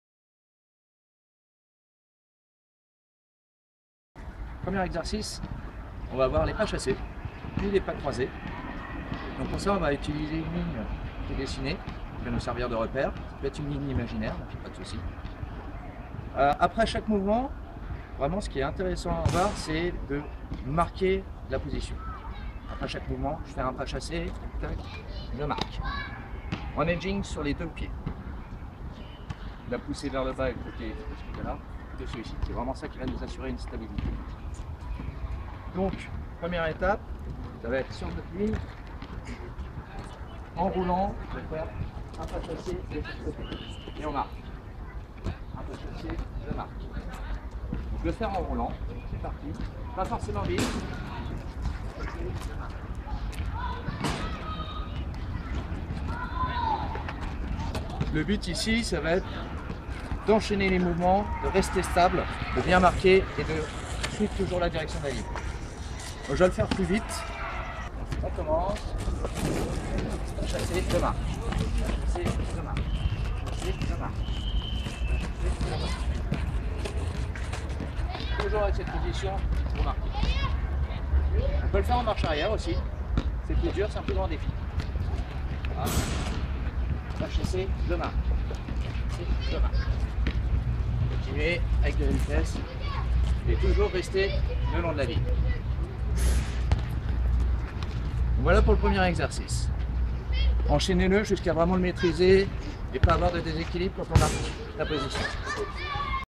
[LES CONSEILS D'UNCLE BENCH] Exercice d'agilité n°1 : Pas chassé
Premier exercice pour travailler les pas chassés.
Exercice pouvant être pratiqué n'importe où, n'importe quand, en solo ou en petit groupe...

Définissez vous un objectif avant de commencer :
- distance de la ligne droite
- nombre d'aller-retours
- sens de patinage (en avant, en arrière, alterné)

Objectif n°1 : garder sa stabilité
Objectif n°2 : exécuter le chassé vite en gardant la stabilité
Objectif n°3 : enchaîner vite le pas, le marquage puis l'autre pas

N'accélérez pas la cadence tant que l'objectif précédent n'est pas bien maîtrisé

Exemple de séance :
20 aller-retour sur une distance de 50m en alternant marche avant et marche arrière à chaque longueur.

30 aller-retour sur une distance de 50m avec une transition avant/arrière au milieu de la longueur (pour plus de difficulté)

20 aller-retour sur une distance de 50m en marche avant. Mais a chaque fois que je perds de la stabilité , je recommence a zéro.